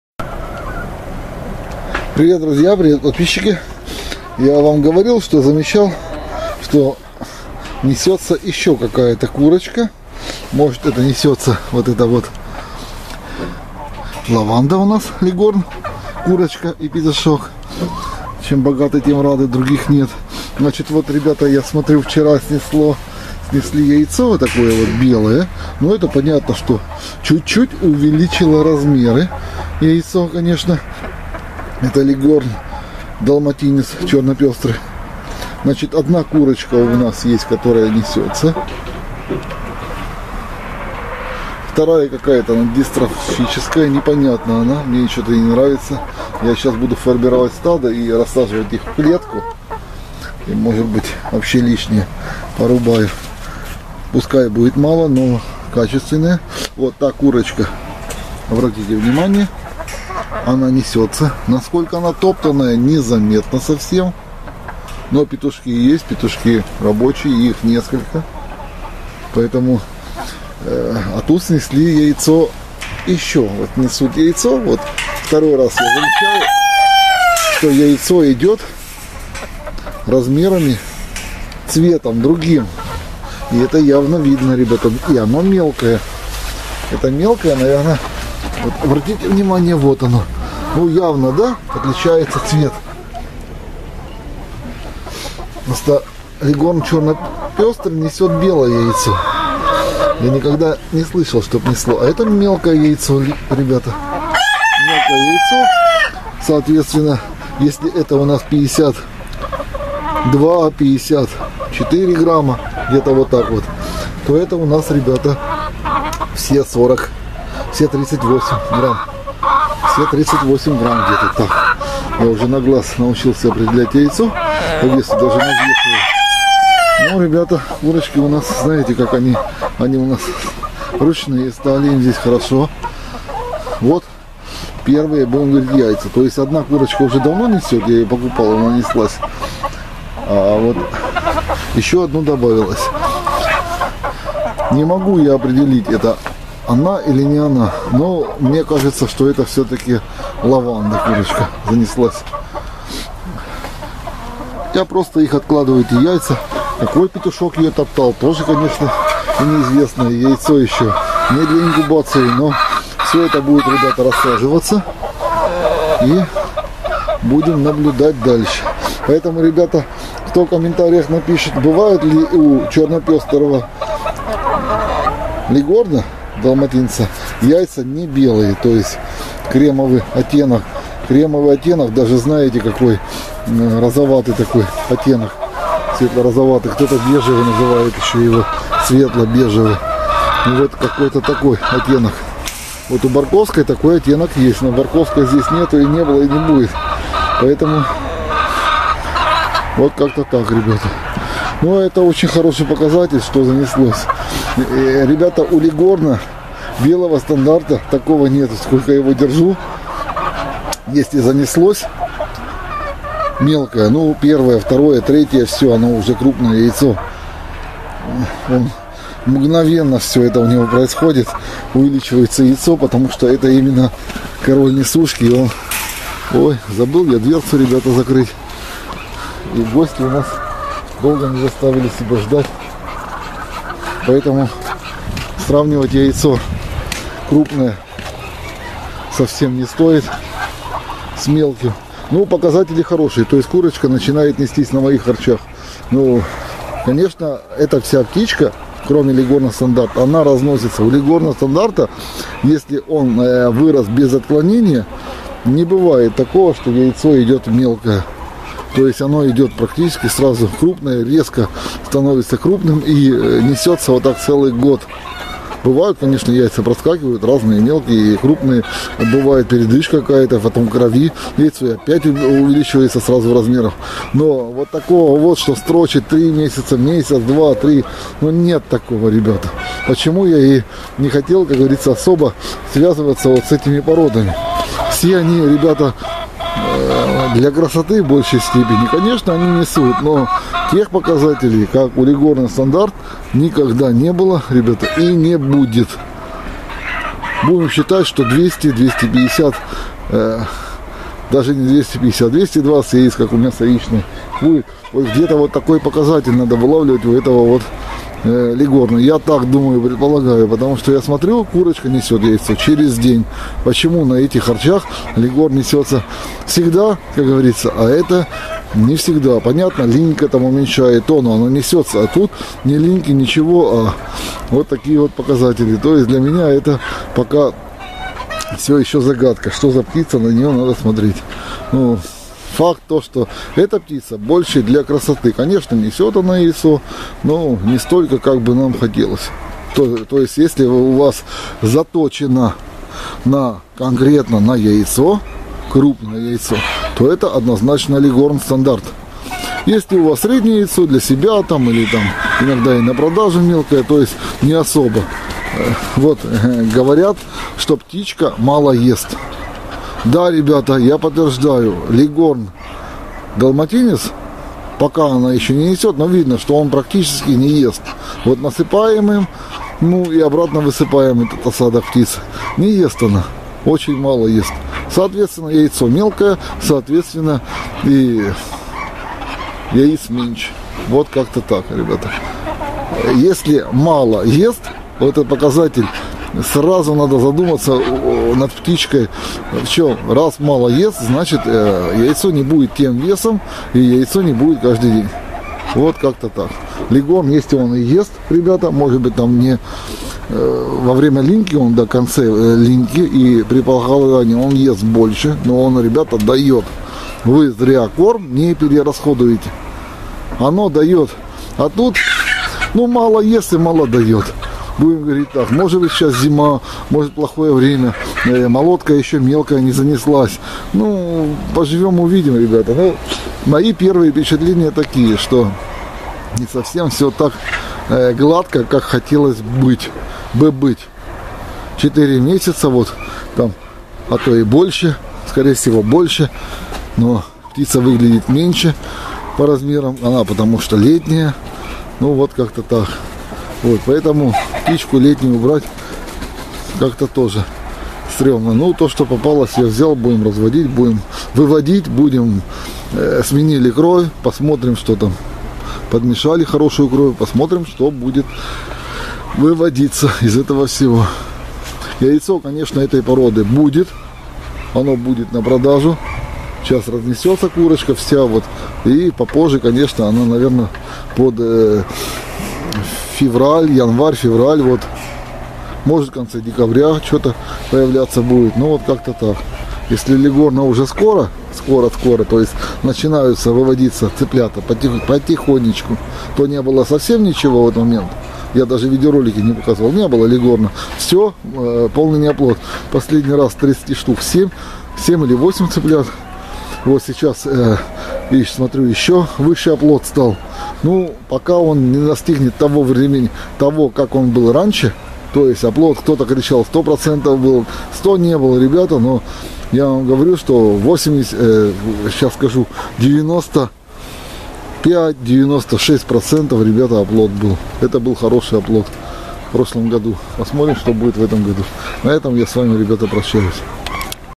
Кто снёс яйцо
Ролик про цветных Леггорнов и первое яйцо, не известно от кого.